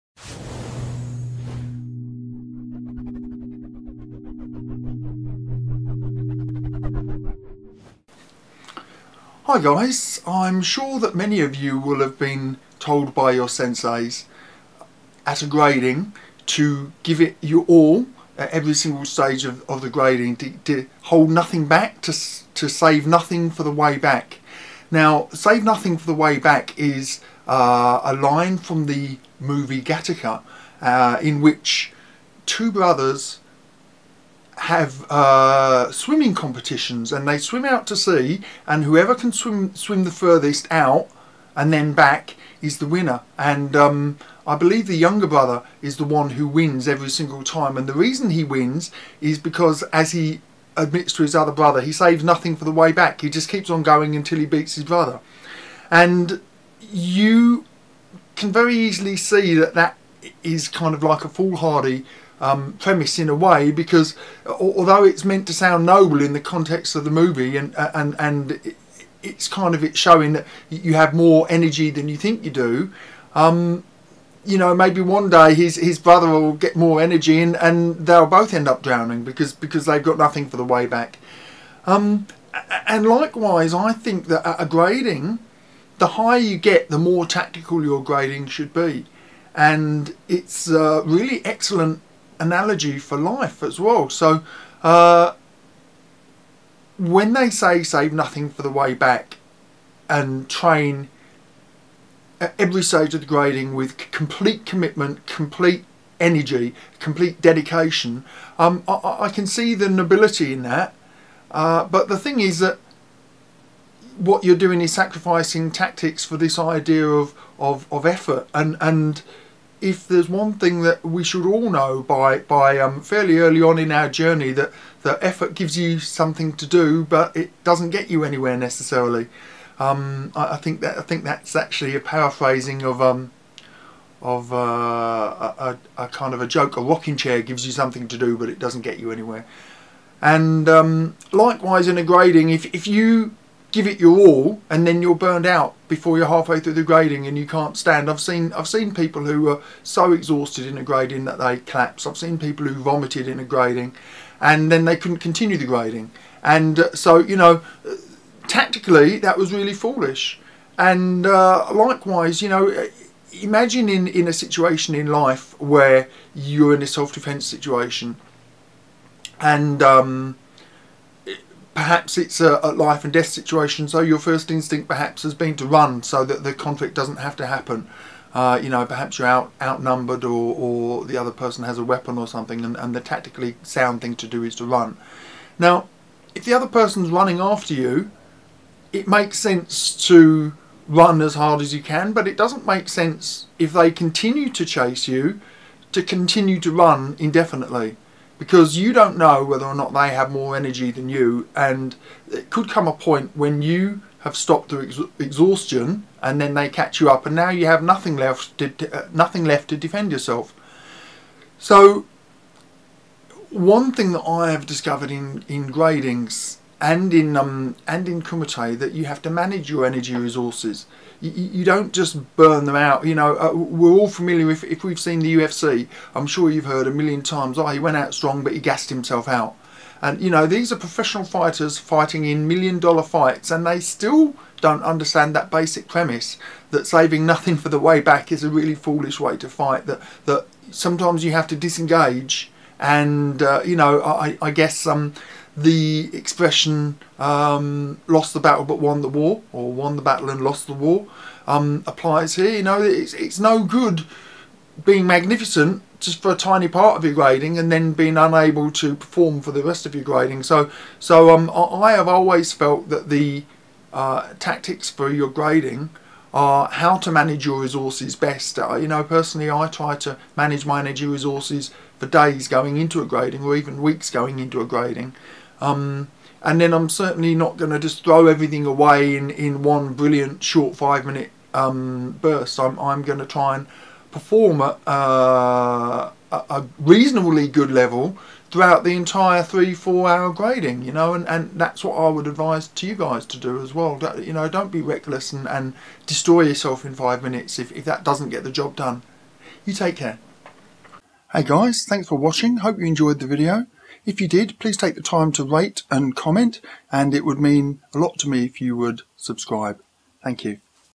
Gradings - Save something for the way back
A reckless approach to your energy management during a grading is neither noble, nor does it show karate maturity.

Watch this video before your next grading!